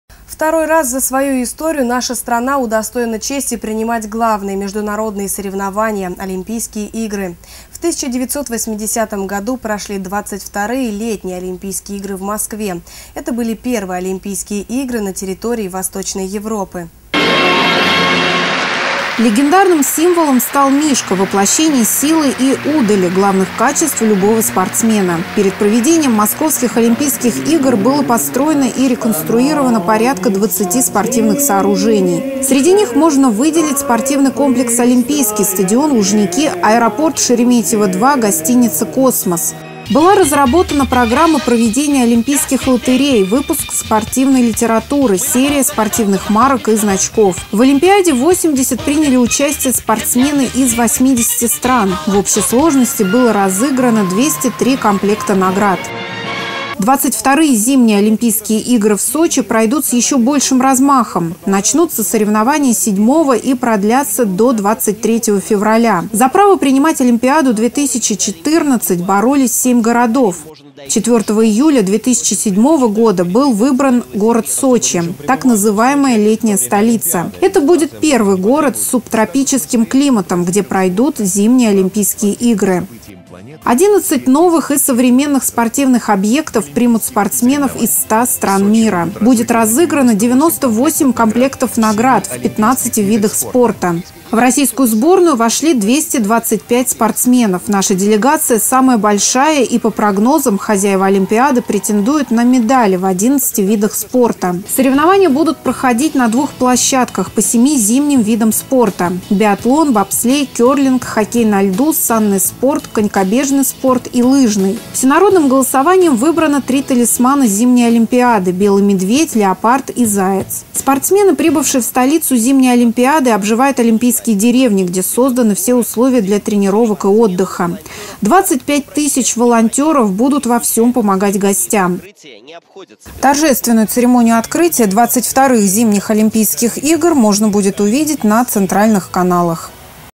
Зимние Олимпийские игры 2014
Второй раз за свою историю наша страна удостоена чести принимать главные международные соревнования -- Олимпийские игры. В 1980 году прошли 22 летние Олимпийские игры в Москве. Это были первые олимпийские игры на территории Восточной Европы.

Наталья Киселева